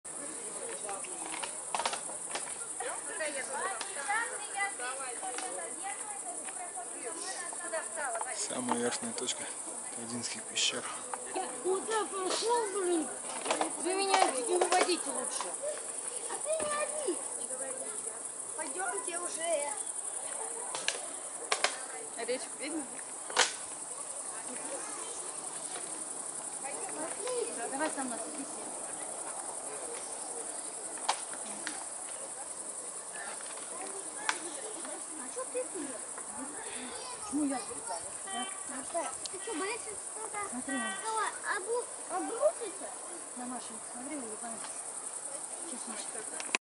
Алтай, Тавгинские пещеры вид на реку Катунь ч.5
Project "Life-Travels"
Алтай, Тавгинские пещеры вид на реку Катунь часть 5.Тавгинские пещеры в окрестностях Бирюзовой Катуни. Протяженность пещер составляет порядка 5 километров, они являются наиболее посещаемыми пещерами в республике Алтай.
Altai Tavginskie cave view of the river Katun part five. Tavginskie caves in the surrounding area Turquoise Katun. The length of the cave is about 5 miles away, they are the most visited cave in the Altai Republic.